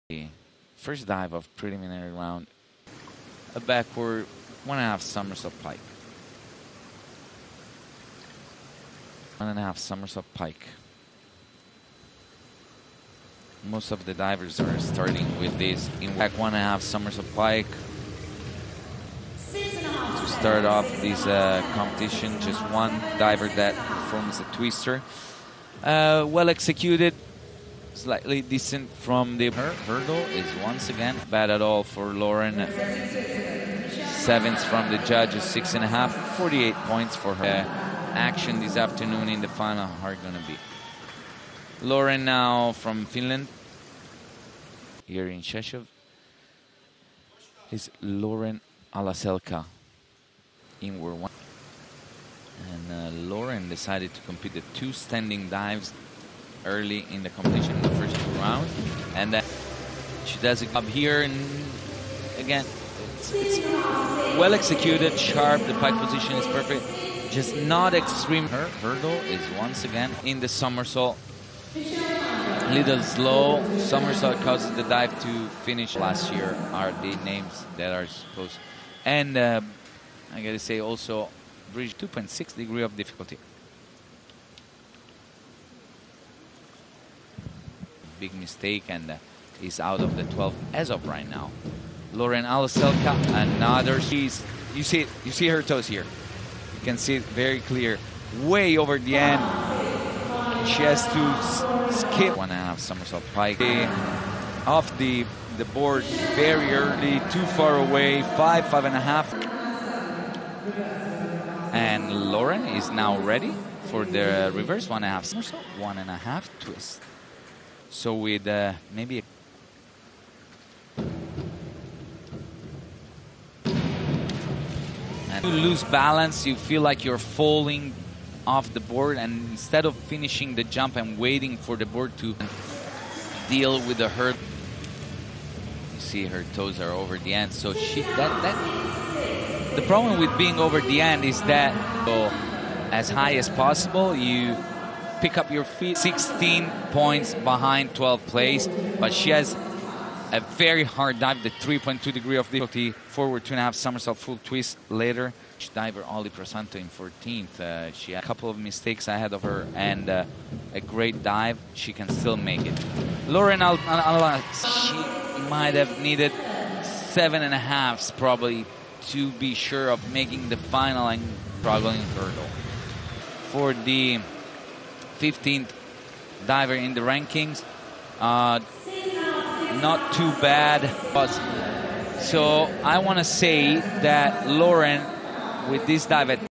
Lauren Nicole Hallaselka (FIN) | 1m Springboard | Women's Diving European Game Krakow
Preliminary for Lauren Nicole Hallaselka in Women's Diving European Game Krakow, Malopolska Poland 2023